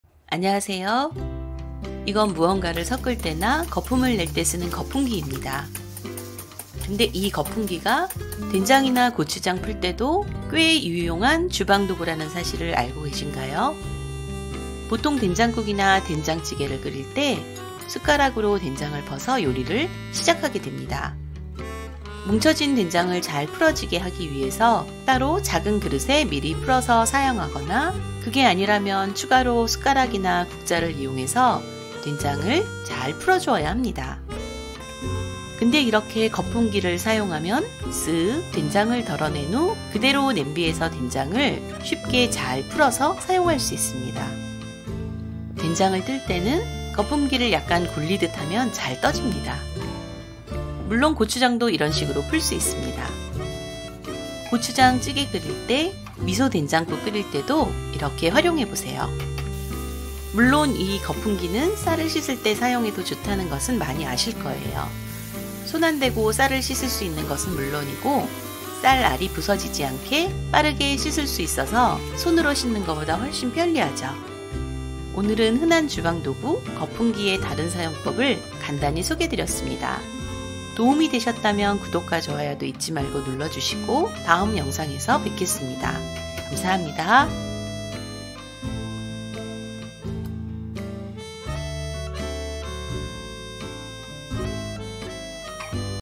한가지 주방도구도 야무지게 써보자!! 흔히 쓰는 주방도구 의외의 활용팁 !이것저것 꺼내지 말고 하나로 다양하게 사용해요👍
무언가를 섞을 때나 거품을 낼 때 쓰는 거품기. 근데 이 거품기가 된장이나 고추장을 풀 때도 꽤 유용한 주방도구라는 사실을 알고 계신가요?
고추장찌개 끓일 대 미소된장국 끓을 때도 활용해보세요